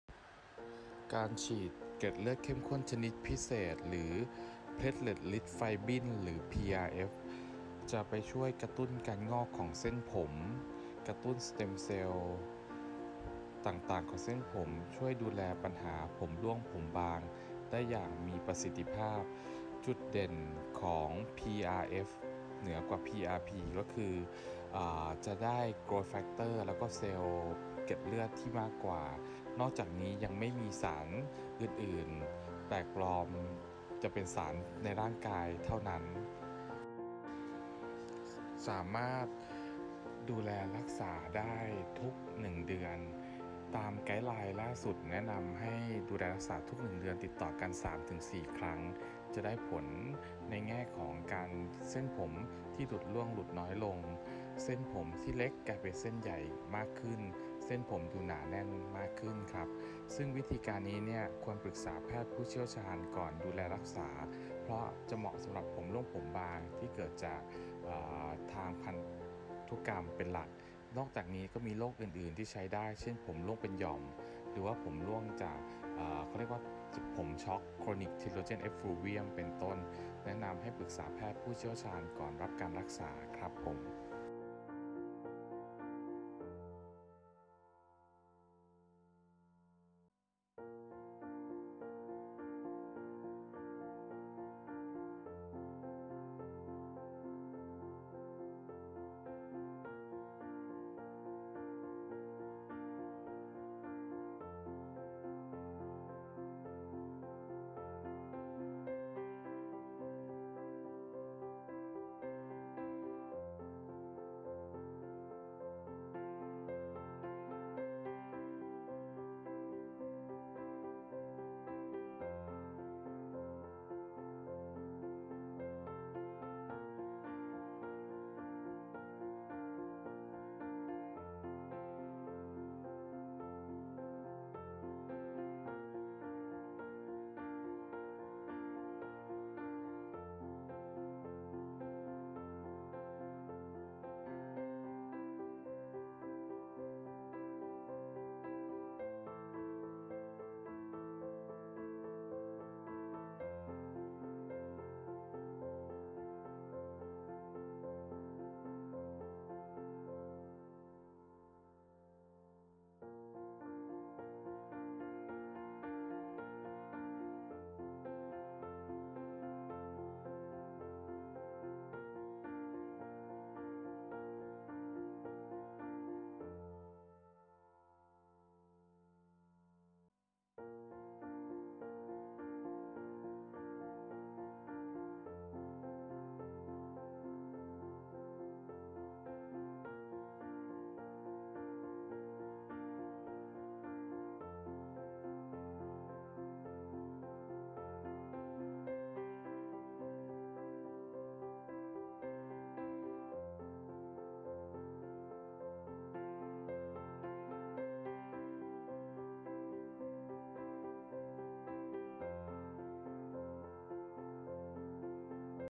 Premium PRF Platelet-Rich Fibrin เกล็ดเลือดเข้มข้นชนิดพิเศษ สุดยอดเทคโนโลยีดูแลผมร่วงผมบสง
Premium PRF Platelet-Rich Fibrin เกล็ดเลือดเข้มข้นชนิดพิเศษ สุดยอดเทคโนโลยี Growth factor กระตุ้นการงอกของเส้นผม ดูแลรักษา ผมร่วง ผมบาง

✔️ ช่วยกระตุ้นการงอกของเส้นผม ช่วยรักษาภาวะผมบาง ด้วยสารกระตุ้นการเติบโตของเซลล์
✔️นวัตกรรมใหม่ส่วนประกอบของ เลือด เข้มข้น เทคนิคเฉพาะของเดอเมชคลินิกเท่านั้น
✔️ที่สุดของความงามอย่างเป็นธรรมชาติ นวัตกรรมที่เหนือกว่าเกล็ดเลือดเข้มข้นทั่วไป PRP ด้วย PRF
✔️ เทคโนโลยีจากอเมริกา
✔️ อีกขั้นของประสิทธิภาพ และระยะเวลาการออกฤทธิ์กระตุ้น บำรุงผิว
✔️ ใช้เลือดของคนไข้เอง มาผ่านกระบวนการพิเศษ สะกัดได้สาร GrowthFactors หลากหลายชนิด ที่ช่วยกระตุ้นการเติบโตของเซลล์
- การรักษาร่วมกับ Laser Hair Cell Stimulation จะช่วยให้ผลการการรักษาผมบางดียิ่งขึ้น
-พิเศษ Red Laser Hair Regrow (LLLT) แสงเลเซอร์สีแดงกระตุ้นรากผม กระต้นการงอก ลดการหลุดร่วง หลังการรักษาด้วย PRF

👉ลดเลือนริ้วรอยคืนความอ่อนเยาว์ให้ใบหน้าและลำคอ
❇️ปรึกษา สอบถามเพิ่มเติม: ❇️